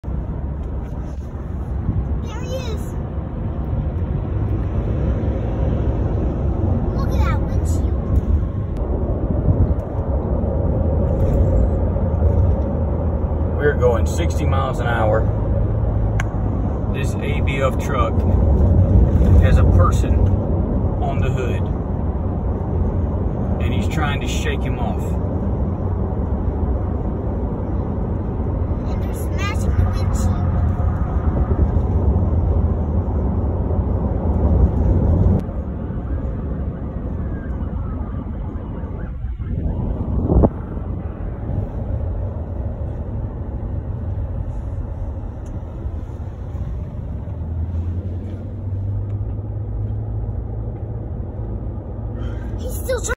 Man Clings to Truck at Highway Speed || ViralHog
Occurred on August 1, 2020 / Florida, USA

"My Daughter Whitney and I were traveling north on the Florida Turnpike around mile marker 86 when we passed a semi-truck that had slammed on the brakes. When we passed we saw that a man was on the hood of the truck smashing the windshield with his fist. We slowed down and the truck passed us doing 60+ mph with the guy on the hood. My daughter said in the video, here he comes.  We called 911 and the FHP pulled them over several miles down the road as we passed them again. There were several attempts by the driver of the semi to shake the guy off the hood as we followed behind."